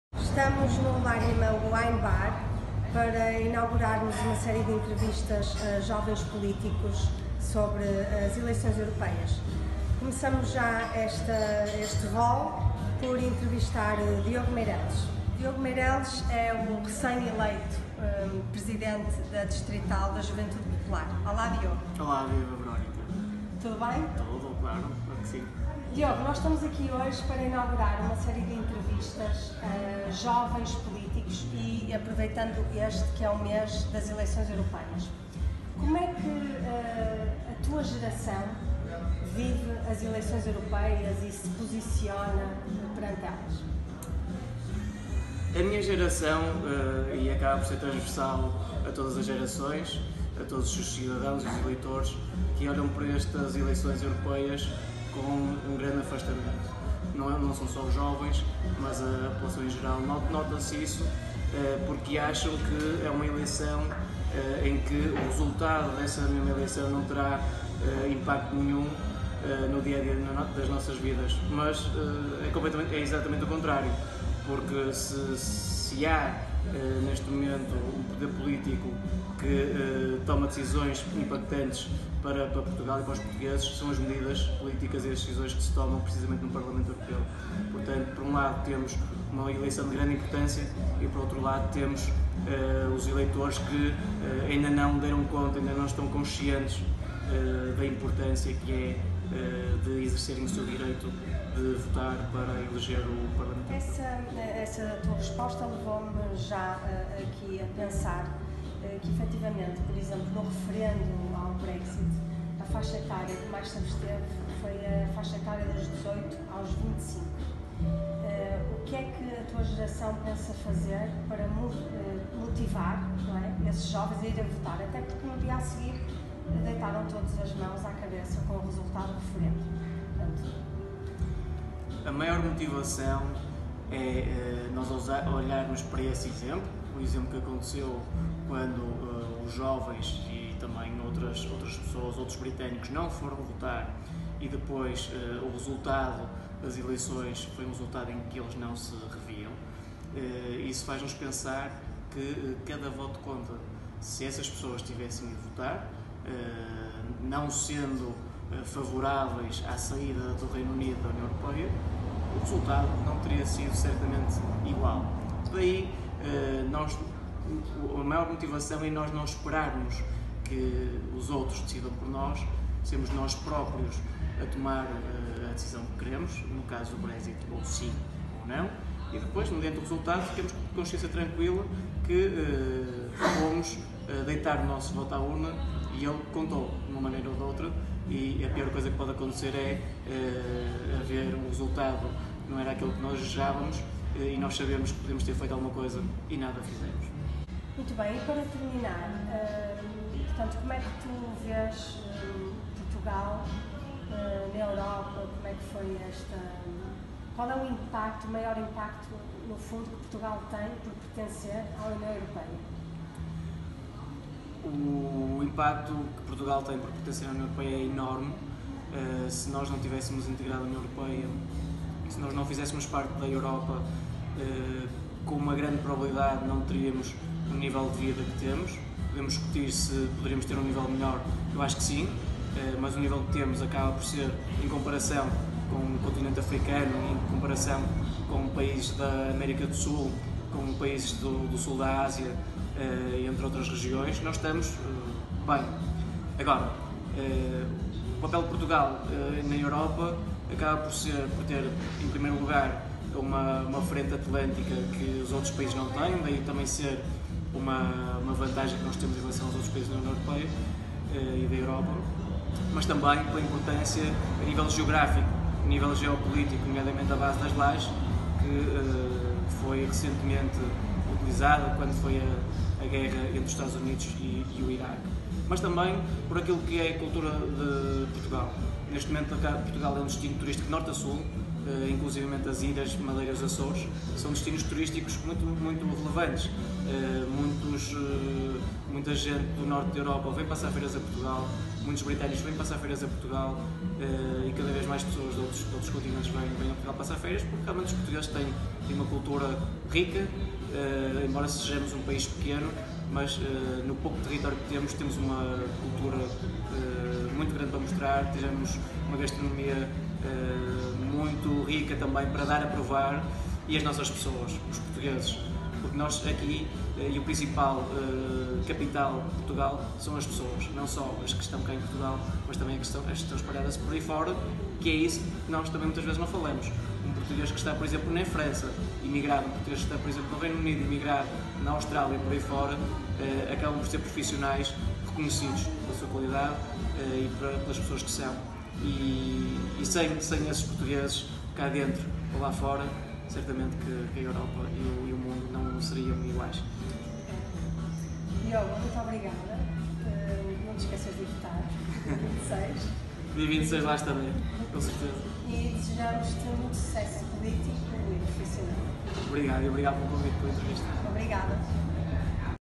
Europeias 2019 - Jovens Politicos
Entrevista a Diogo Meireles .
Apresentamos a nova rubrica de interesse público e cívico: lemagz_civicinterest.
Aproveitamos o mês das eleições europeias e publicamos, a primeira entrevista a um jovem político. Diogo Meireles é presidente da distrital do Porto, da Juventude Popular, tem 27 anos, é licenciado em economia pela Universidade Católica do Porto e Mestre em Gestão pela mesma instituição.